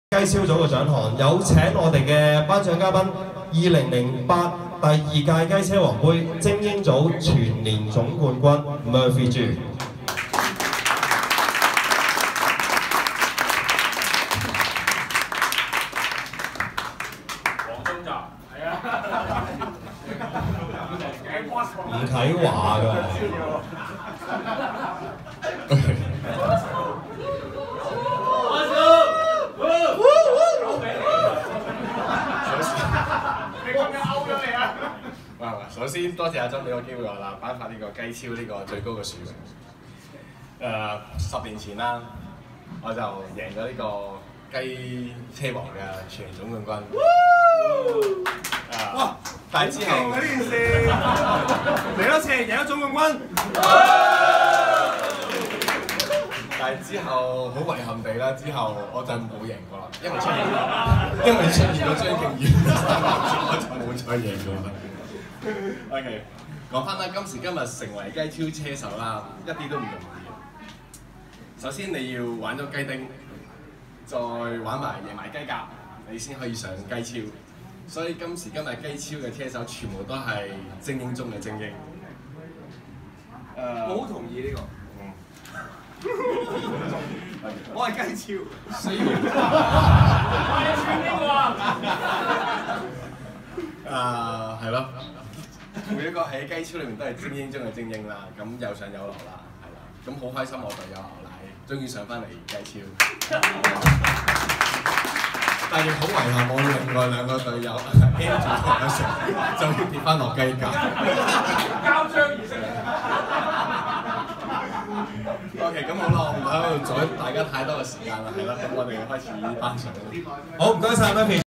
2017-2018 第十一屆雞車王盃頒獎典禮 24/37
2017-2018 第十一屆雞車王盃頒獎典禮  24/37

雞超組頒獎嘉賓:
Murphy Chu (2008 精英組全年總冠軍)

***特別鳴謝豹哥拍心口全晚幫手錄影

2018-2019 雞車王盃全年七站
首站9月16星期日三水

即日開始接受報名

詳情請參閱

雞車王
圓一個賽車手夢